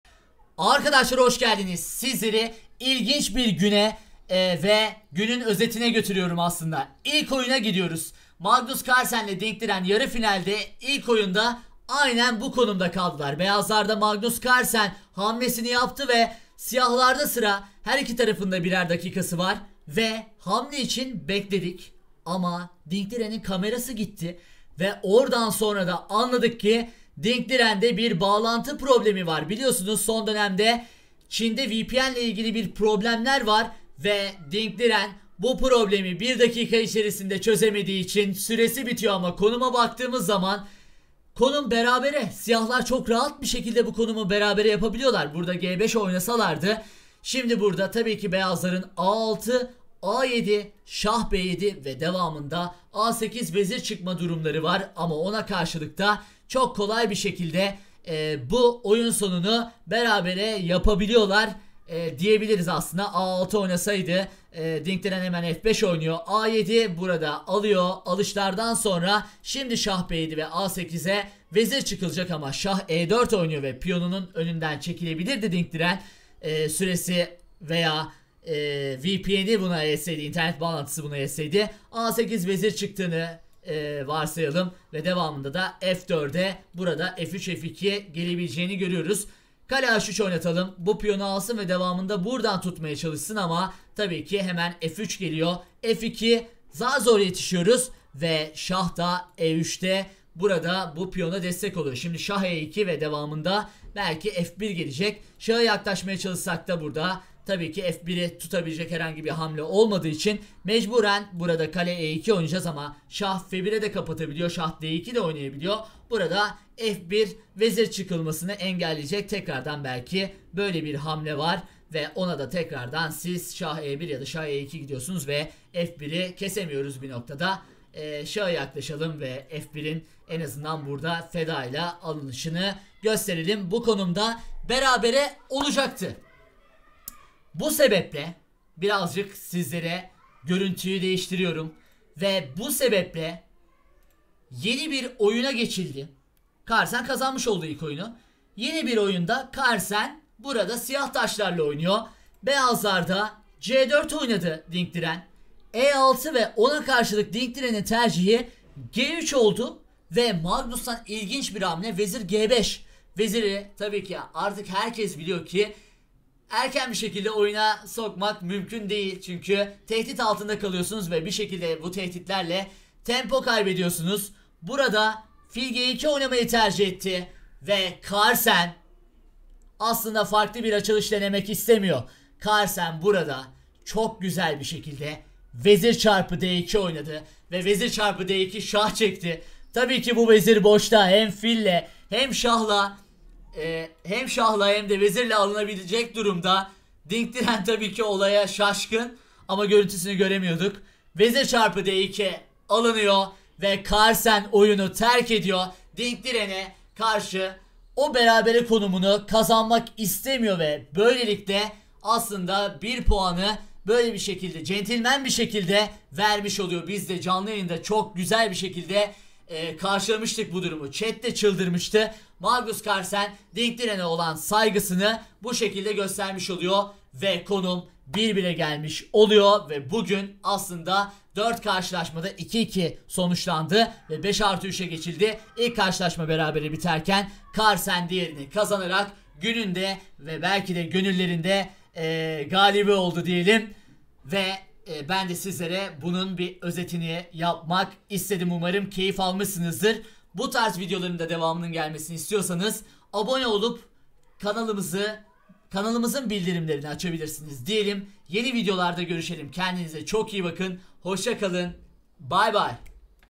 Allah Allah Carlsen Niye 4. Hamlede Terk Etti? 3. Hamlede Vezir Fedası! Ding Liren- Magnus Carlsen
Magnus Carlsen Açılışta Vezir Feda ediyor desek az bile söylemiş oluruz, 3. hamlede vezir feda eden Carlsen'in belki de hayatı boyunca en kısa oynadığı maçı 4. hamlede sonuçlandırıyor. Peki NEDEN?